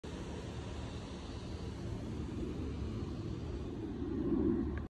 Cedar privacy fence with beveled rot/base board completed in Humble,TX
Humblefencecompany.net